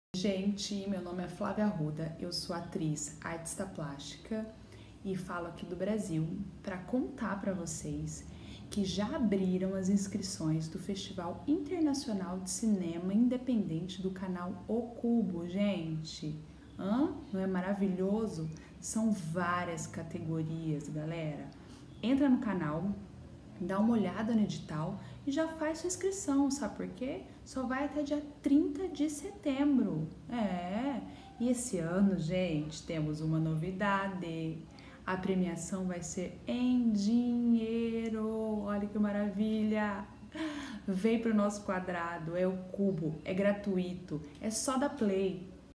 Flávia Arruda dá o recado! EDITAL ABERTO!
Nossa atriz, Flávia Arruda, que faz a vilã BIA no filme EU TE AMO RENATO veio aqui dar um recadinho para todos! Diz ae Flavinha!

ABERTAS AS INSCRIÇÕES para o VII FESTIVAL INTERNACIONAL O CUBO DE CINEMA INDEPENDENTE EM LÍNGUA PORTUGUESA!

Este ano com MUITAS novidades e PREMIAÇÕES em dinheiro! 🎬🎬🎬

O Edital completo e a ficha de inscrição você pode baixar em nosso canal

COMPARTILHE!